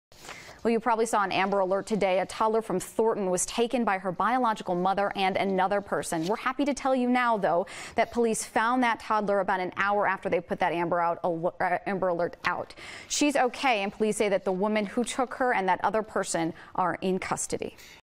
Amber Alert canceled for 2 year old
A toddler from Thornton was taken by her biological mother and
another person. Police found that toddler about an hour after they put out the Amber Alert.